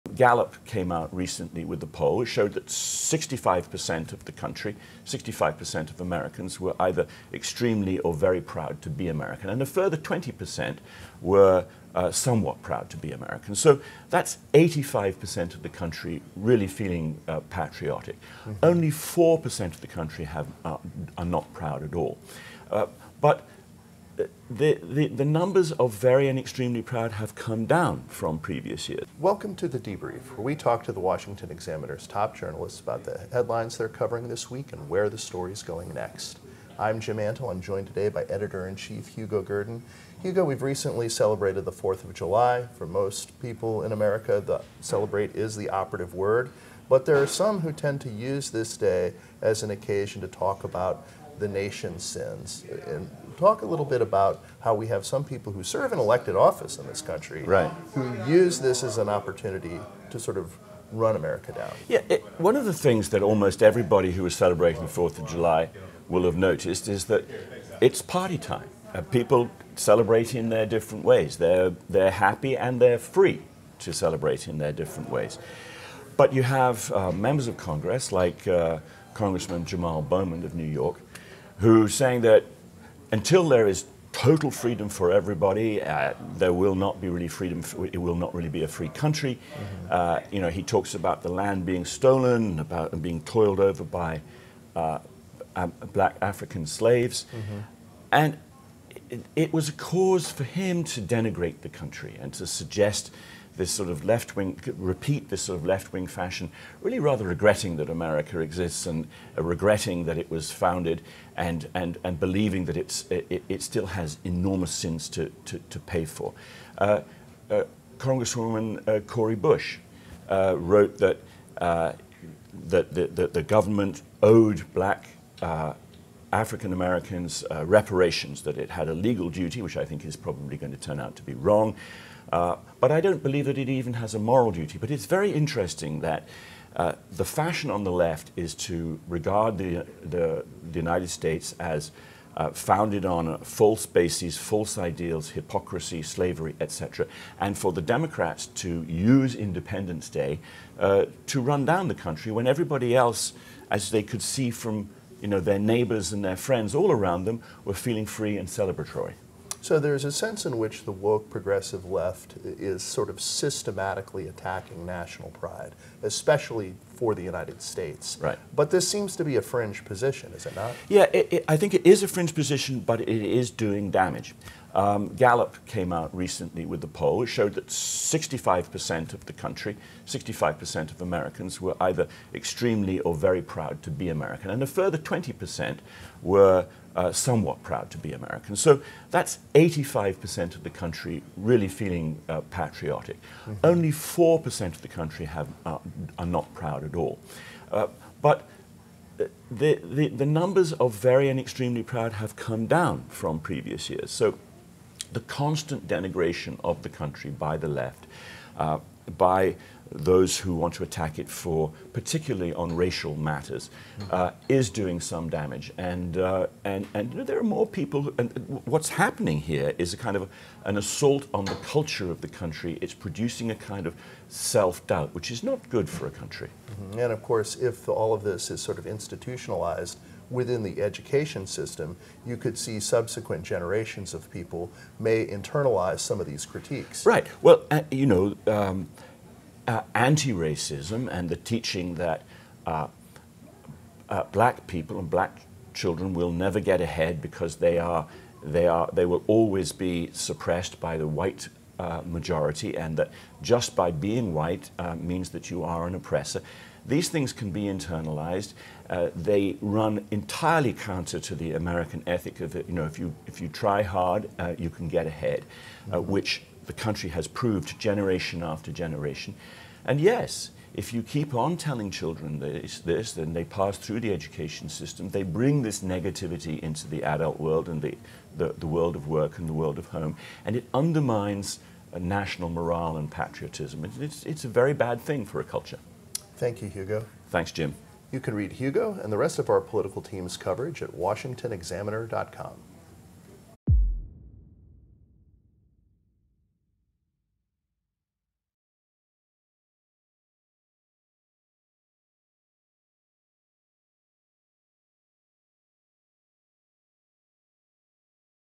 The Debrief: Hugo Gurdon on why Americans should have pride in patriotism
Editor-in-Chief Hugo Gurdon joins Politics Editor Jim Antle to discuss how the Fourth of July should be a time of celebration but, instead, some progressive members of Congress are trying to quash that with fringe ideas.